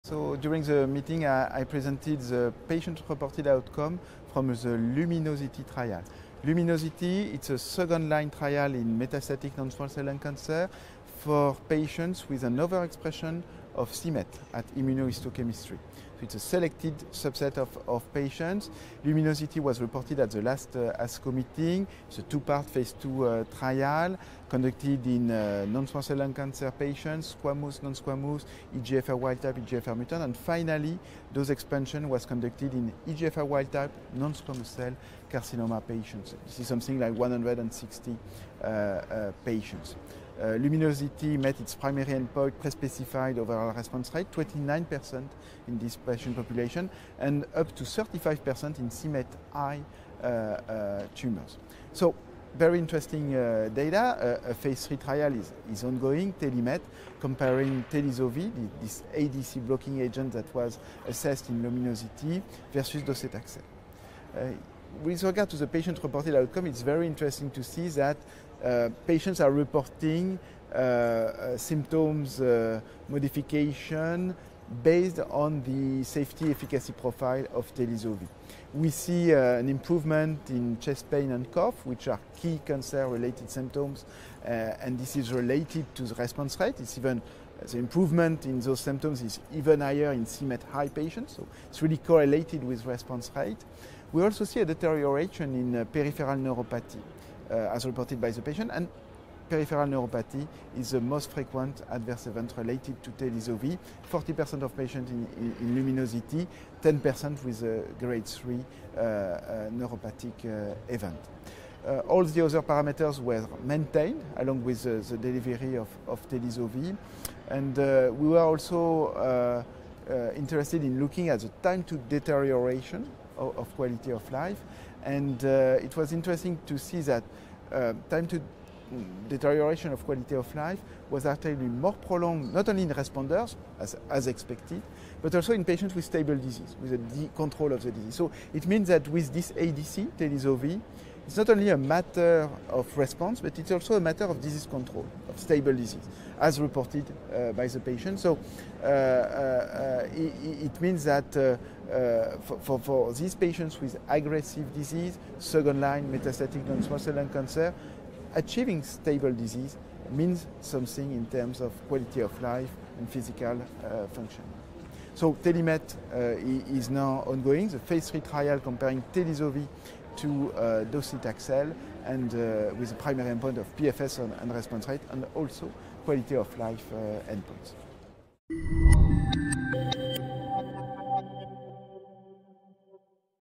Patient reported outcomes for telisotuzumab vedotin in c-Met protein overexpressing EGFR WT nons...
Dr Nicolas Girard speaks to ecancer at ESMO 2024 about the patient reported outcome results from the LUMINOSITY trial. This trial evaluated telisotuzumab vedotin in patients with c-Met protein overexpressing, EGFR wildtype, non-squamous non-small cell lung cancer.

Lung cancer/treatment-related symptoms and impact on function and quality of life were evaluated using the EORTC quality of life questionnaires.

Dr Girard reports that patients treated with telisotuzumab vedotin reported improvement or maintenance in most patient-reported outcomes evaluated.